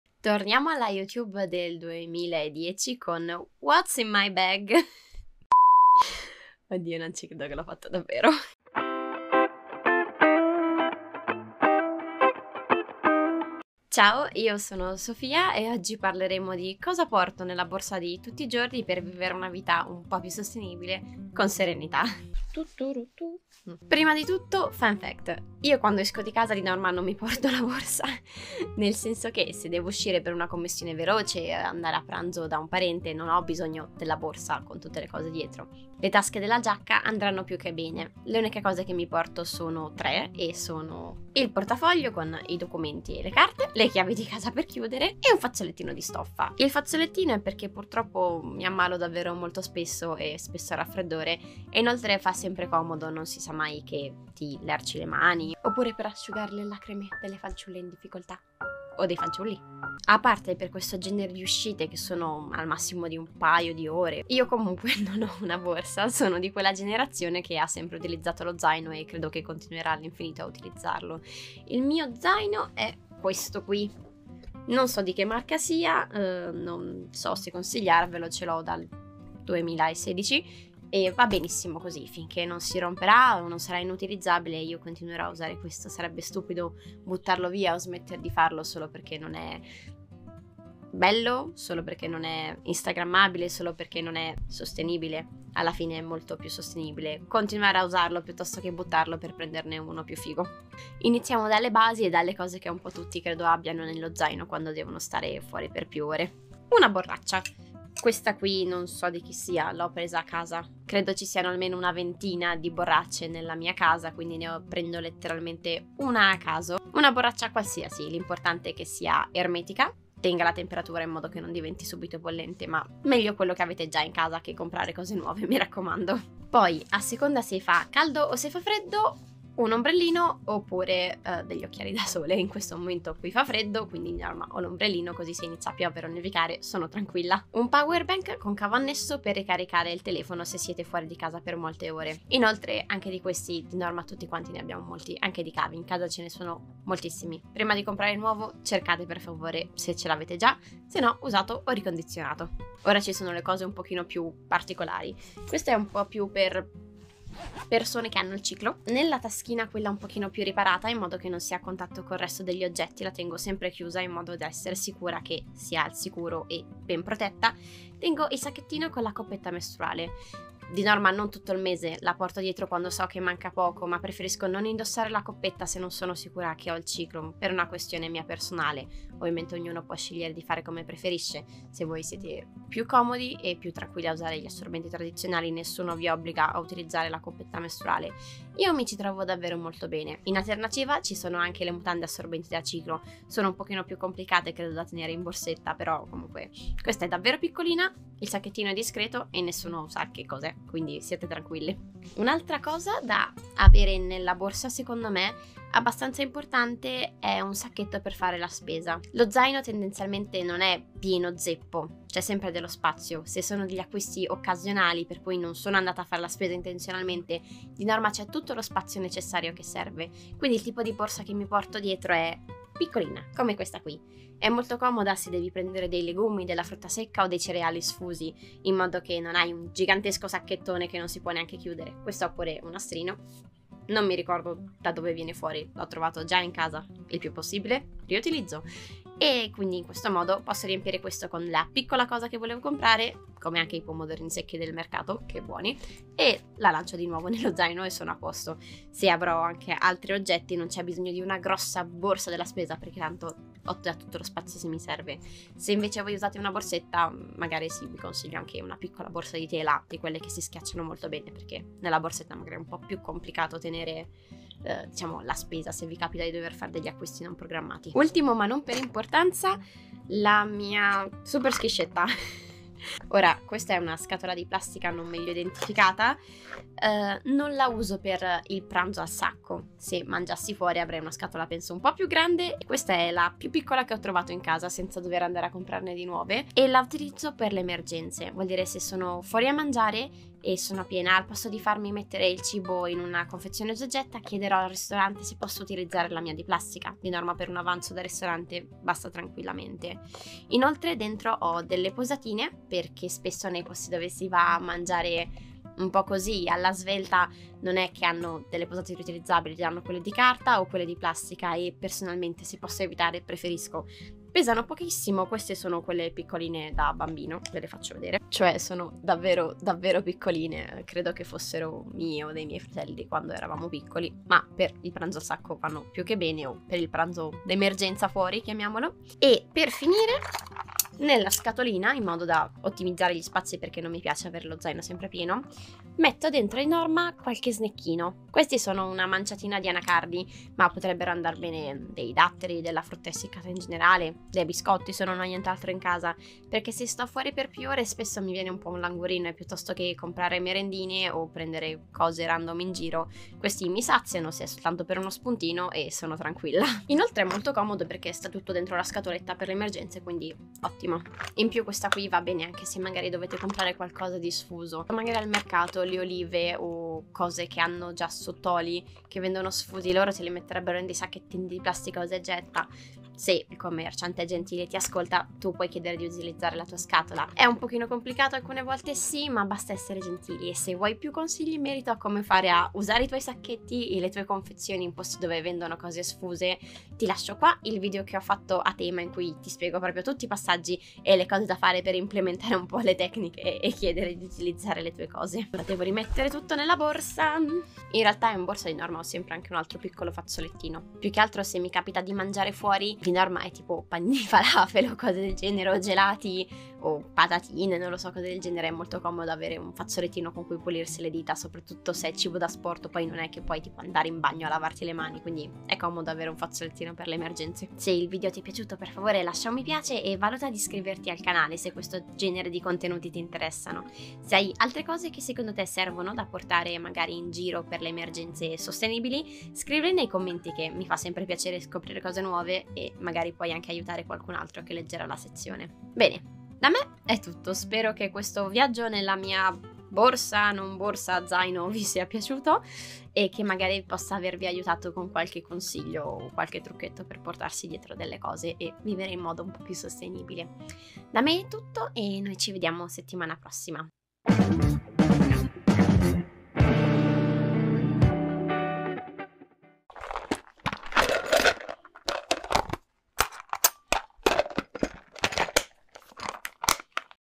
What's in my bag? ESSENZIALI zero waste | Essere (un po' pù) Sostenibili
Ho amato questo genere di video quando ero più piccolina e ricrearlo con i miei "Essenziali (un po' più) Sostenibili" mi ha davvero divertita, spero anche a voi!

00:00 Intro
00:25 Niente borsa
01:07 Zaino
01:43 Le basi
02:34 Il Kit Sostenibile: Coppetta mestruale
03:28 Il Kit Sostenibile: Sacchettini per la spesa
04:30 Il Kit Sostenibile: Super schiscetta
05:36 Il Kit Sostenibile: Snack
06:06 Chiedi di usare i tuoi contenitori
06:58 Conclusione
07:40 Papere 🤣

*Video* Come chiedre di utilizzare il tuo sacchetto?

*I miei Essenzili da Borsa* _(da notarsi l'ironia)_
- Portafoglio: quello che già avevo (se no, second hand)
- Portachiavi: quello che già avevo (se no, second hand)
- Fazzoletto: quello che già avevo (se no, second hand)

- Zaino: quello che già avevo (se no, second hand)

- Borraccia: quello che già avevo (se no, second hand)
- Powerbank: quello che già avevo (se no, second hand)
- Ombrello: quello che già avevo (se no, second hand)

- Coppetta mestruale (questa è quella che ho io e mi trovo bene ma esistono tantissime marche, prima di comprare fai una ricerca per trovare il modello più corretto per te! notsponsored )
- Sacchettino spesa: quello che già avevo (se no, second hand)
- "schiscia": quello che già avevo (se no, second hand)
- Posatine: quello che già avevo (se no, second hand)
- Anacardi e snack vari: dal mio mercato cittadino sfusi!